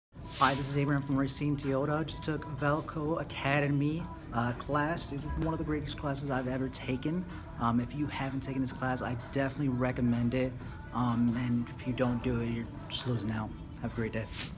VELKO ACADEMY- One of the greatest classes I’ve ever taken
Attendee of Velko Academy’s workshops is endorsing Velko and sharing his experience